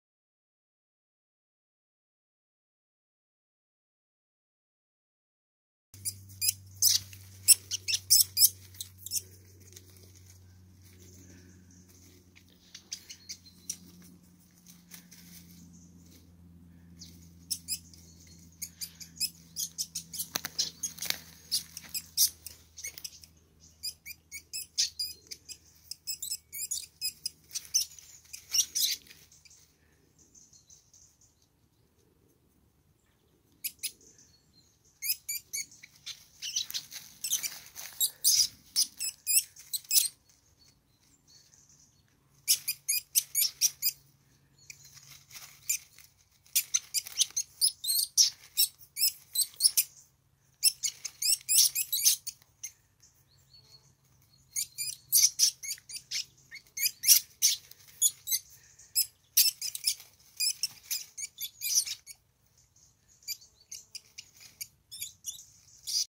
Kampf der Mauswiesel
Die beiden Kerlchen waren so beschäftigt, dass sie mich gar nicht bemerkt haben :)